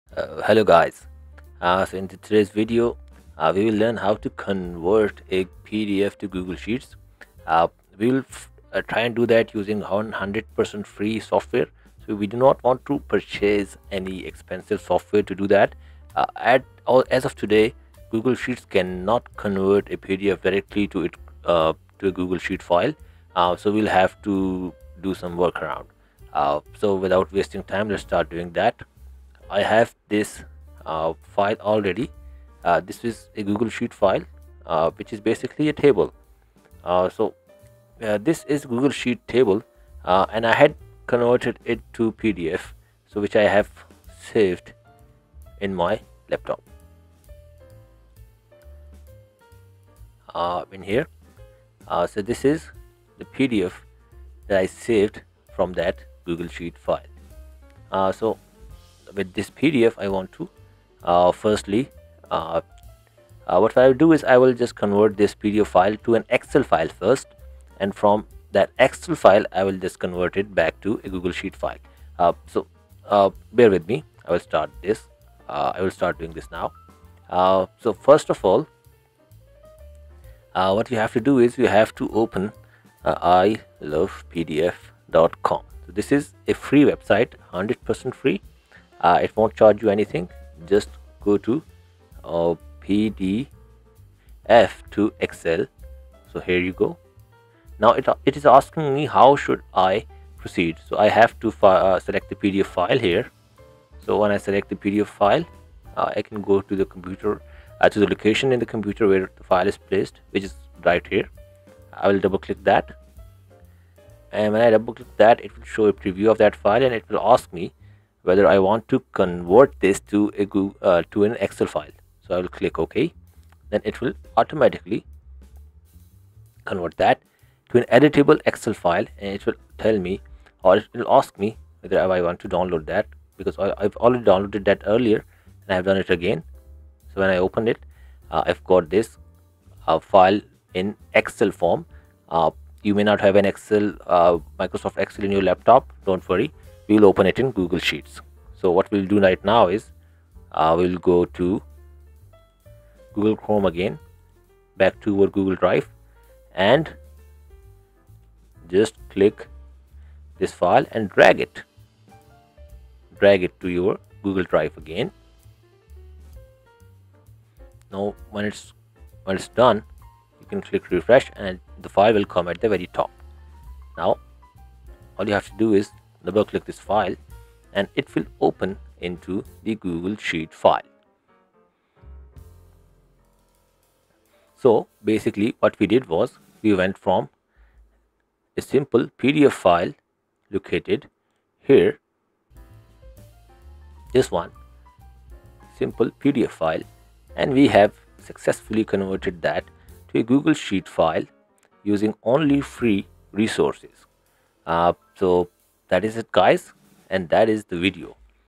How to Convert PDF Table to Google Sheets
In this tutorial, you'll learn how to convert a PDF file into Google Sheets quickly and efficiently. This guide will show you various methods, including using online tools and manual data extraction, to import and organize data from PDFs into Google Sheets for easy editing and manipulation.

Key topics covered:

How to convert PDF to Google Sheets
Import PDF data into Google Sheets
Convert PDF to spreadsheet format
Extract data from PDF to Google Sheets
How to edit PDF data in Google Sheets
This tutorial is applicable for both beginners and advanced users working with Google Sheets on any platform (Windows, macOS, Linux, Chromebook).